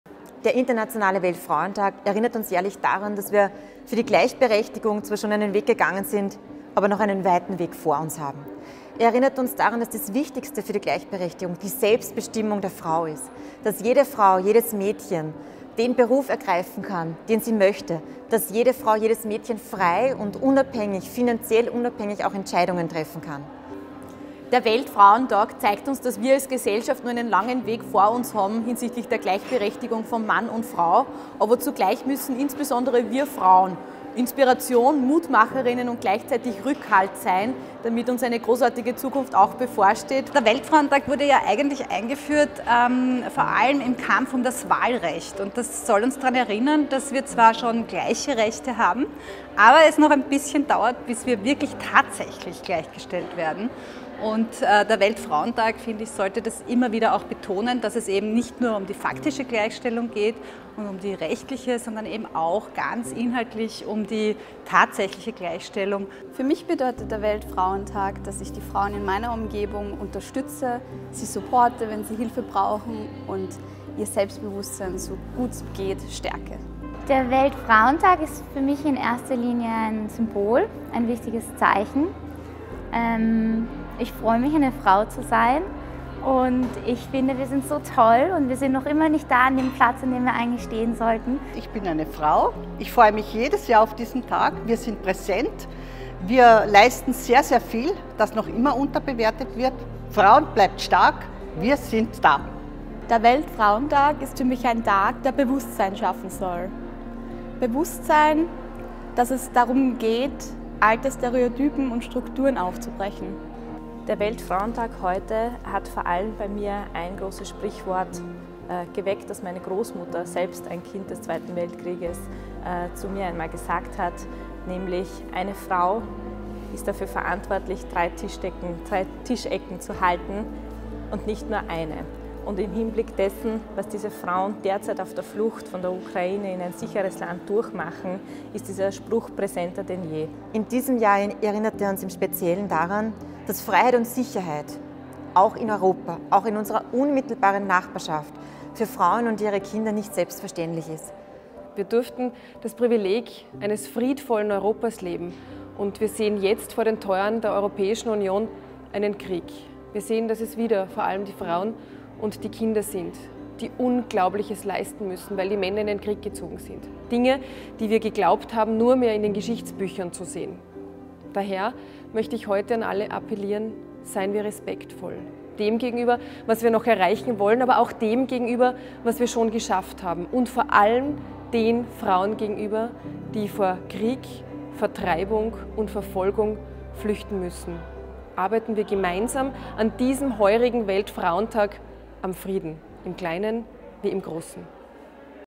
Weltfrauentag 2022 im Bundeskanzleramt
Was bedeutet der Weltfrauentag für dich? Wir haben Frauenministerin Susanne Raab, Europaministerin Karoline Edtstadler, Jugendstaatssekretärin Claudia Plakolm und Mitarbeiterinnen aus dem Bundeskanzleramt gefragt!